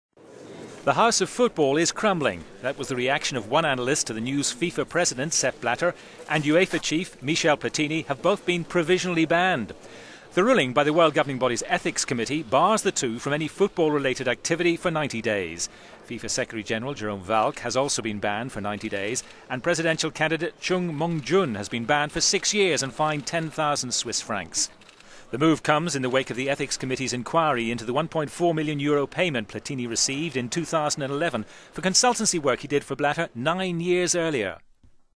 Blatter and Platini banned for 90 days by FIFA's ethics committee
The house of football is crumbling. That was the reaction of one analyst to the news FIFA President Sepp Blatter and UEFA chief Michel Platini have both been provisionally banned for 90 days.

The ruling by the world governing body's ethics committee bars the two from any football-related activity. FIFA secretary general Jerome Valcke has also been banned for 90 days and presidential candidate Chung Mong-joon for six years and fined 10,000 Swiss francs.